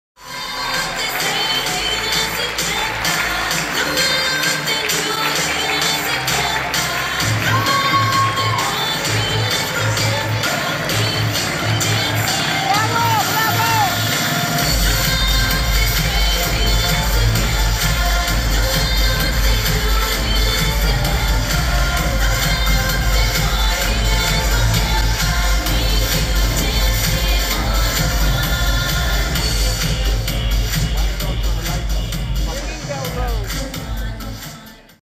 Runway Finale ~ Ringing Bell Robes (Couture Fashion Week Sept. 2013)
Runway finale for Ringing Bell Robes.

Event: Couture Fashion Week
Location: The New Yorker Hotel, New York, NY
Date: September 7, 2013

This is Revenge Fashion Magazine, fashion for all humankind.